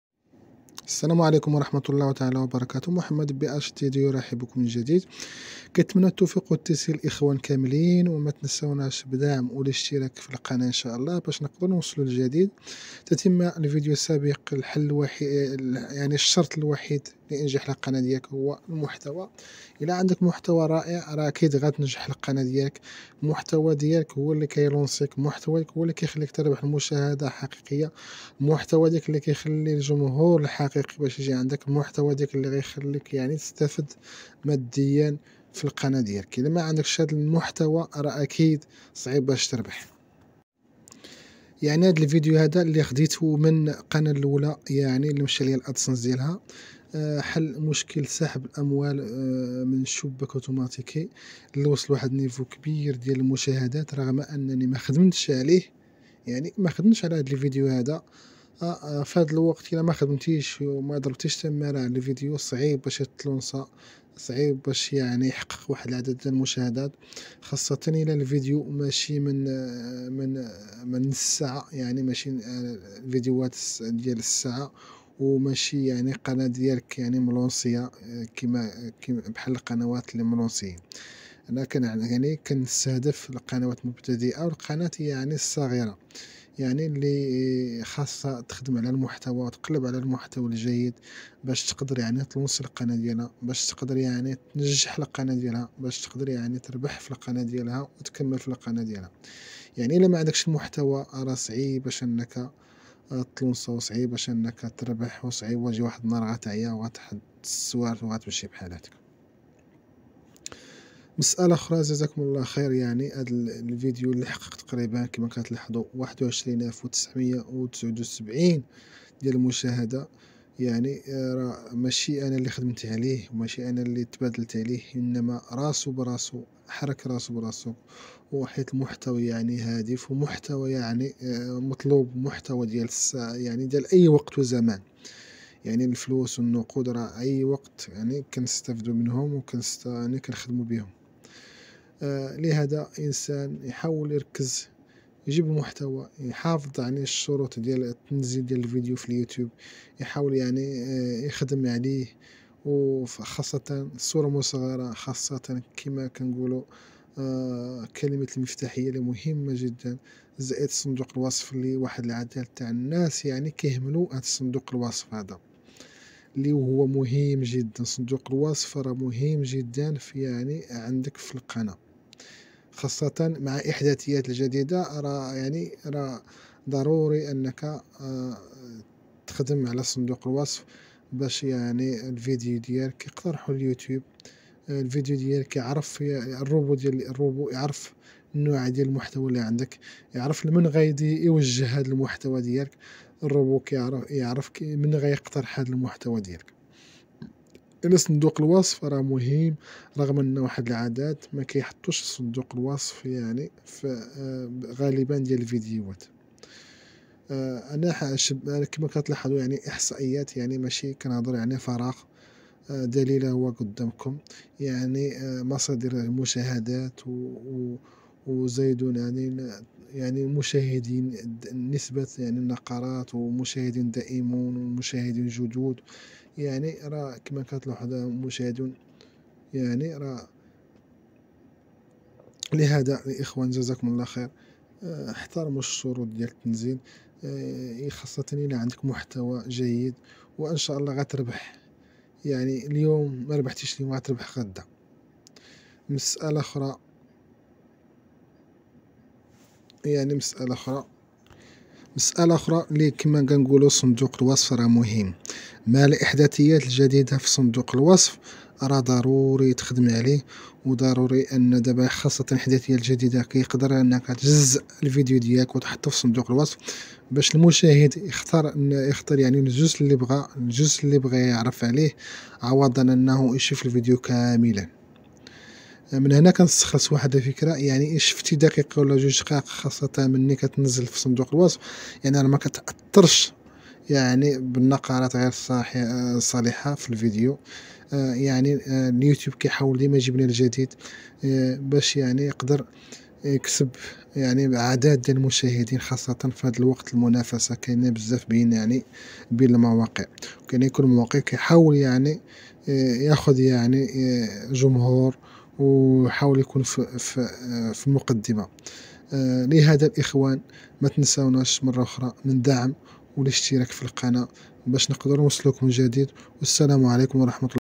كيف يحقق الفيديو أكبر نسبة من المشاهدات#mohamedbhd2
كيف يحقق الفيديو أكبر نسبة من المشاهدات
كيف ارفع نسبة المشاهدة في اليوتيوبثانياً ضبط القناة عنصر فعال في زيادة مشاهدات اليوتيوب

1. إنشاء رمز القناة

2. صورة القناة

3. وصف القناة
المقدمه 00:00
هل الفديو يحقق نسبة المشاهدة مع الوقت 00:45
ماهو المحتوى المطلوب من تحقيق المشاهده 1:56
مشاهده جزاء من الفديو لا يؤثر على القناه 1:5